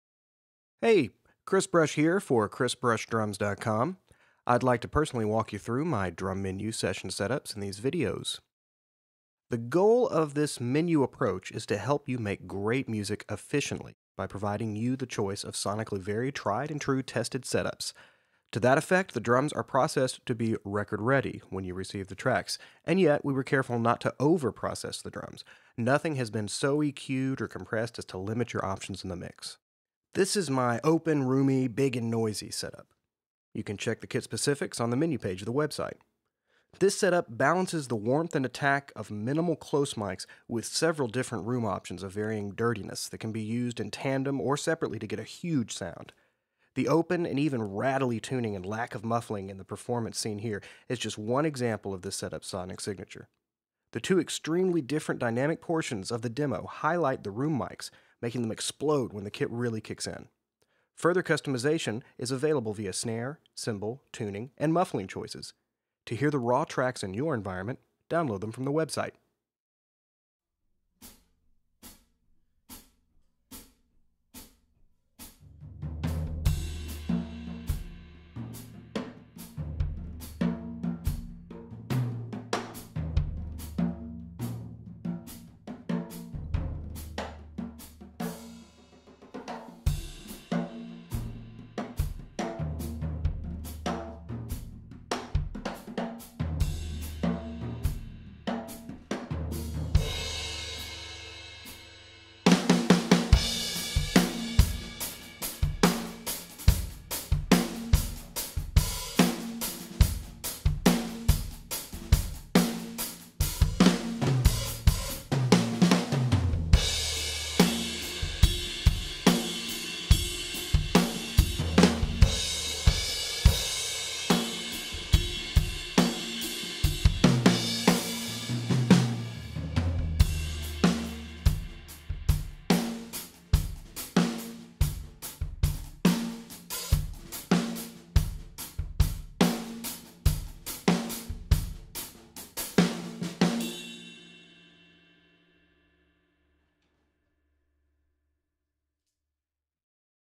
The Chris Brush Drum Menu - Choice D: Open/Roomy/Big & Noisy Setup
Chris Brush Drums - a truly custom drum session experience. Nashville-based drummer Chris Brush explains his Drum Menu concept and how it can help meet your needs for your tracks. This a a video demoing Chris' Open/Roomy/Big & Noisy menu option.